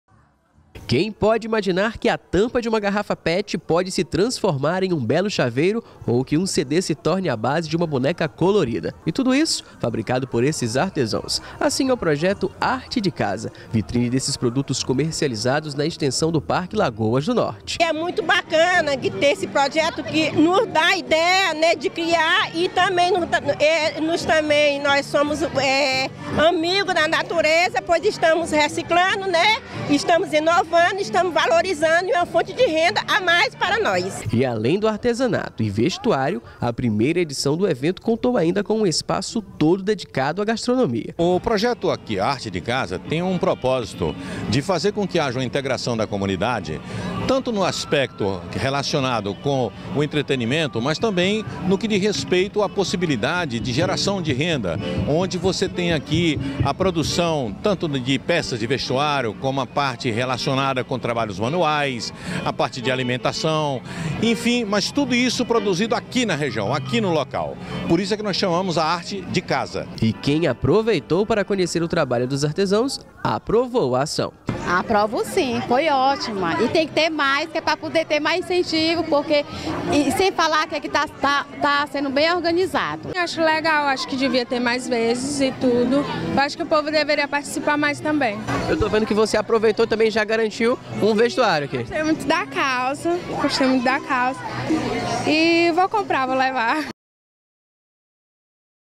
Parque Lagoas do Norte recebe "Arte de Casa - Empreendedorismo que transforma"
O Parque Lagoas do Norte foi o local escolhido para receber a primeira edição do  "Arte de Casa - Empreendedorismo que transforma". O evento, realizado pela prefeitura em parceria com a comunidade, tem como proposta fomentar a economia local com a venda de produtos artesanais. Matéria para o Bom Dia Assembleia e Alepi TV